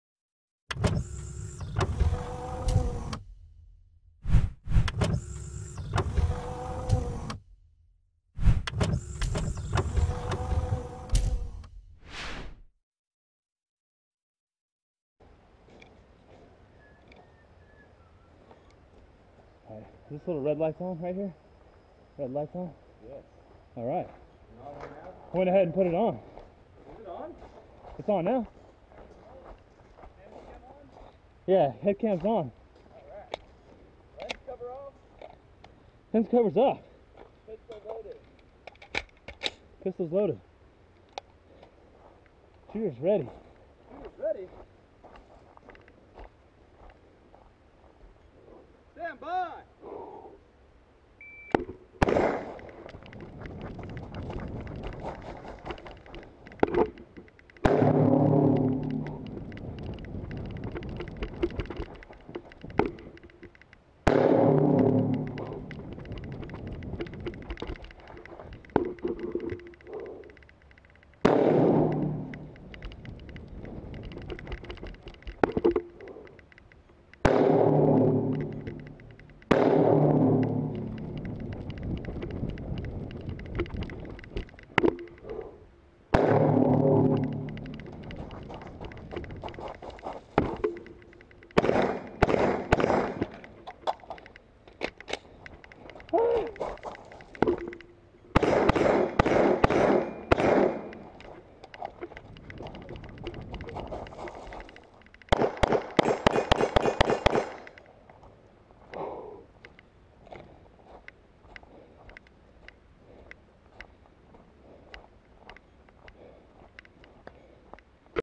RW - Light the fire drill - Contour Camera
3rd time, he forgot to turn it on the first 2